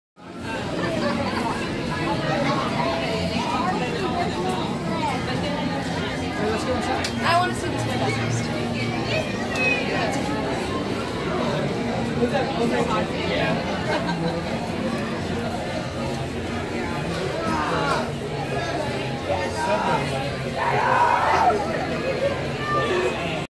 HskyArt Haunted Mansion Halloween window display Disney California Adventure DCA 2018 HSKY
Window display has lots of halloween fun, every window is different